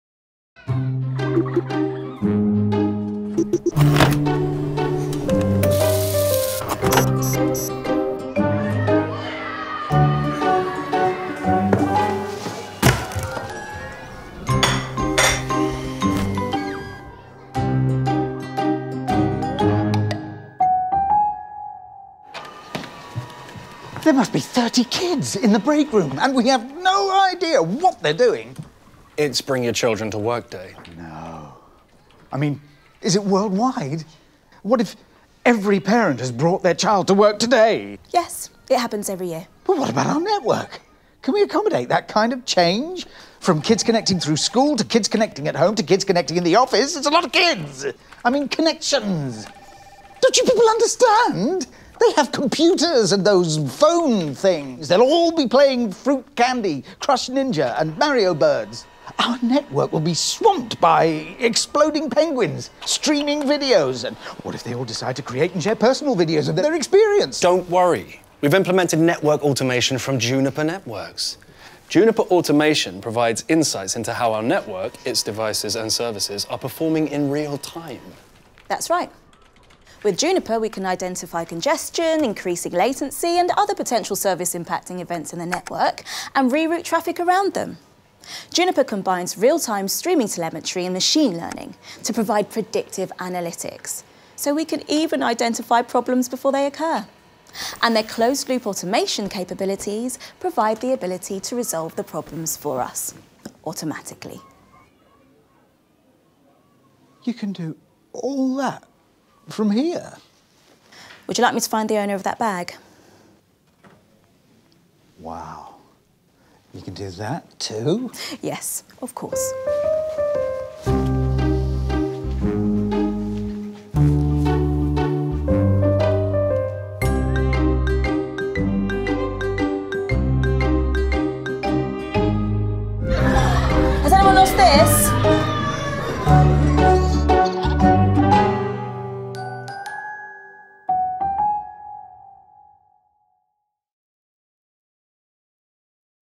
Network Overload
Bob’s network is stressed, and so is he! Should he be worried or is there a solution? Juniper Paragon Automation can destress even the most challenging situations, including Bob’s.